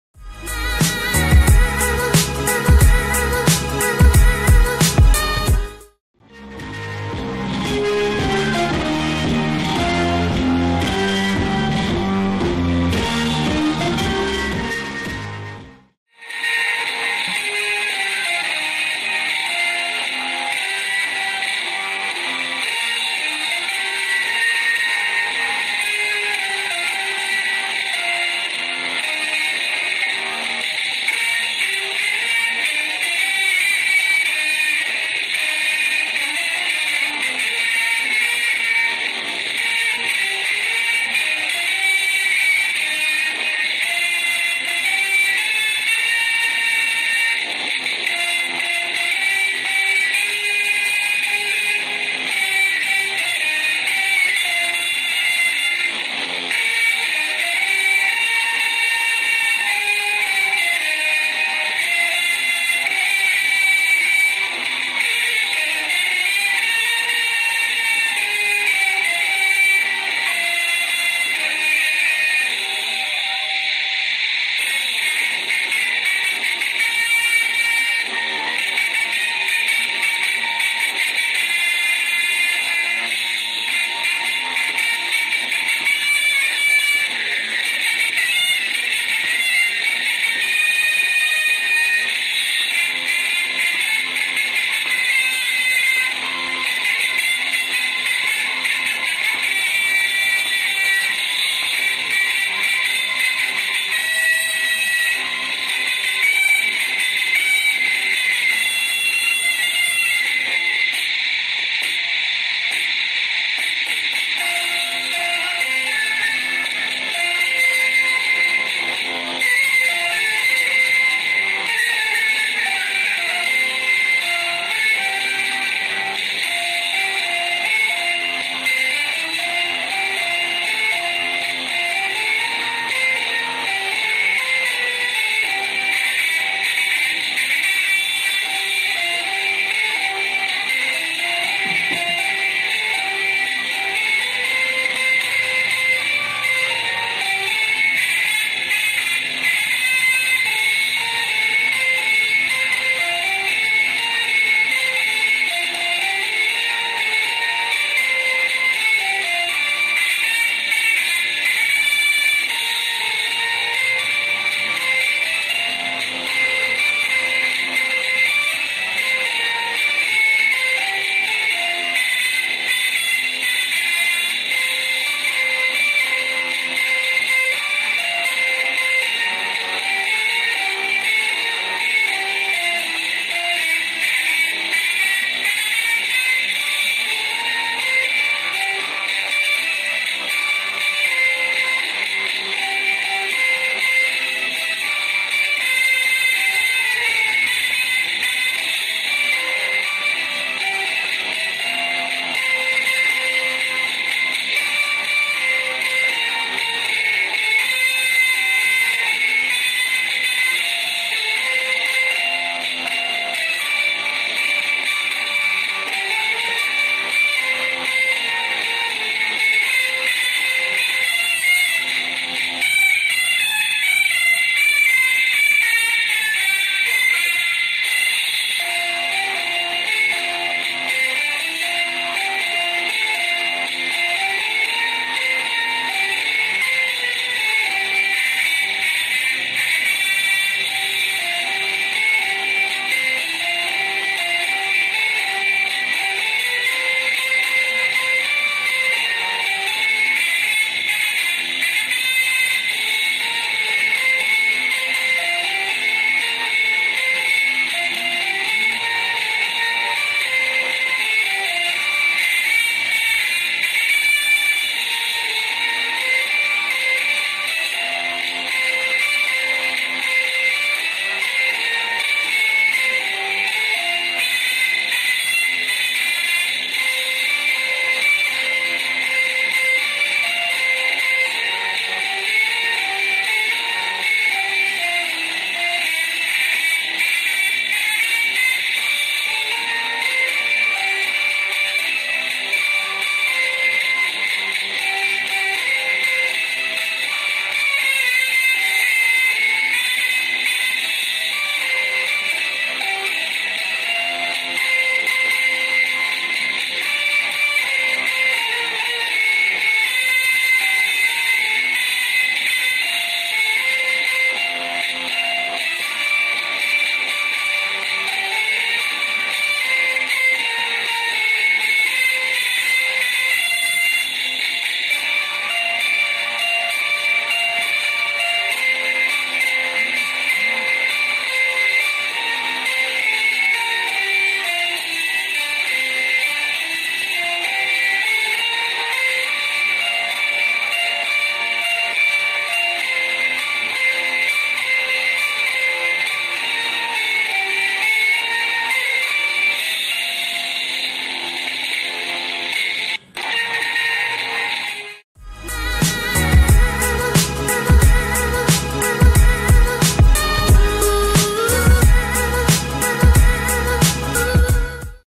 Viralkan! Stop Kebiasaan Alay Dengan Senam Pagi Indonesia Seri D 2021!
Hello, Guys...! Dan salam SKJ, semuanya...! Jangan lupa kepoin terus channel Senam Pagi Indonesia Seri D senam pemersatu bangsa, ya...!

Senam Pagi Indonesia Seri D jelas paling ampun menyetop kebiasaan alay. (Menurut bahasa gaul, artinya alay itu adalah: norak; kampungan.) Mari kita biasakan berolahraga Senam Pagi Indonesia Seri D, tepatnya Senam Pagi Indonesia Seri D 2021, setiap hari mulai sekarang, supaya kita jangan bersikap alay lagi. Semoga bermanfaat...!

Salam SKJ,
SULUNG PAMBRASTO